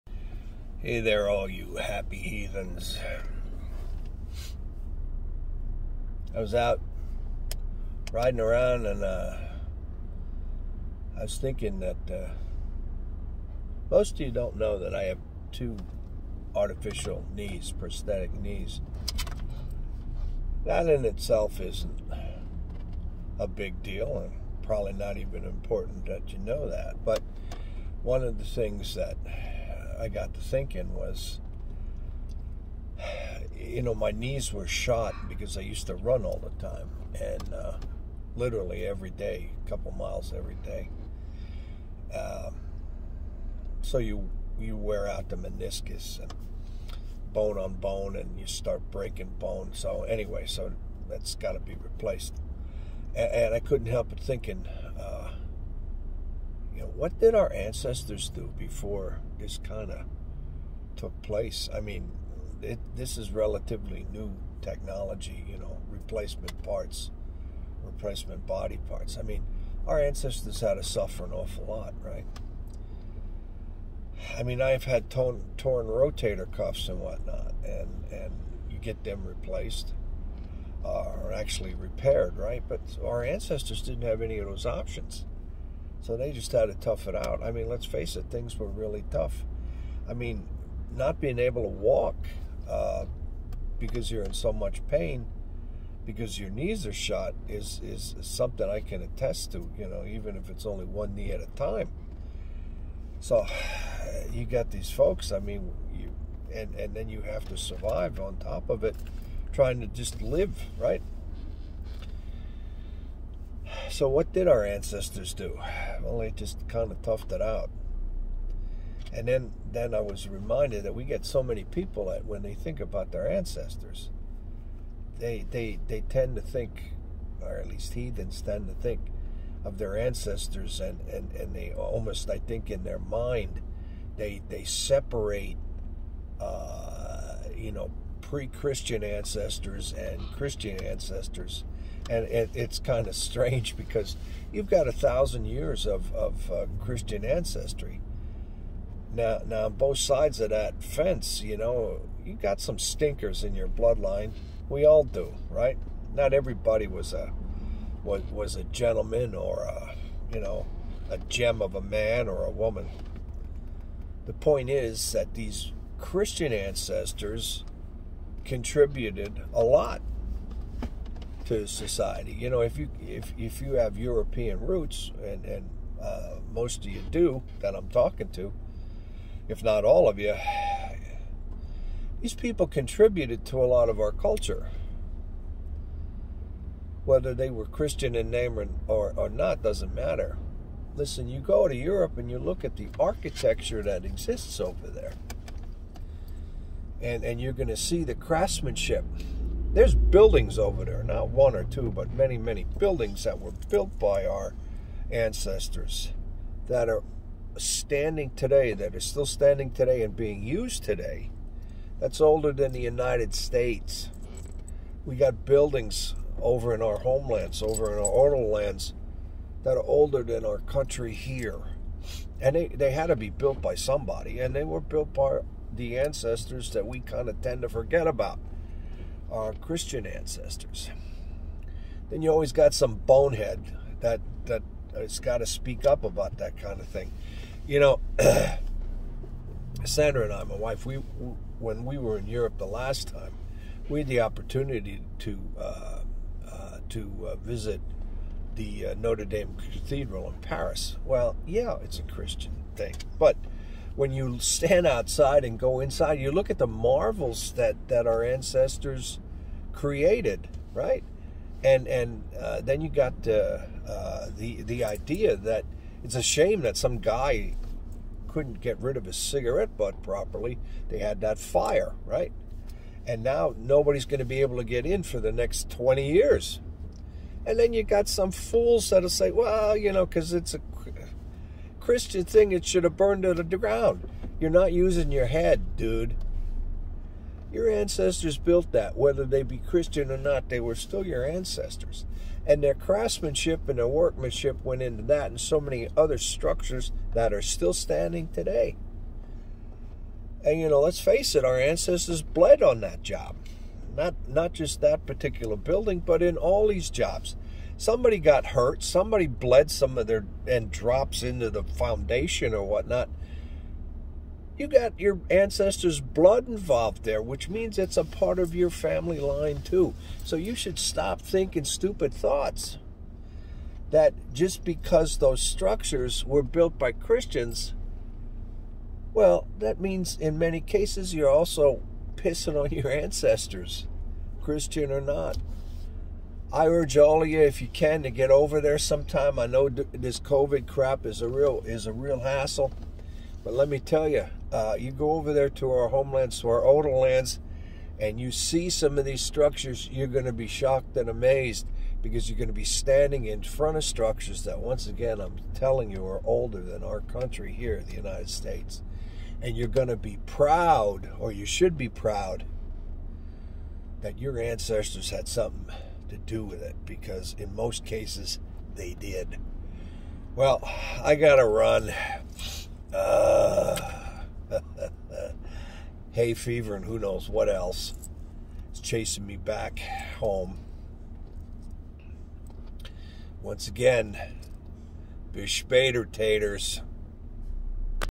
Episode 45. Our Ancestors
Terry talks about our often forgotten Ancestors who grew  up Christian and their accomplishments.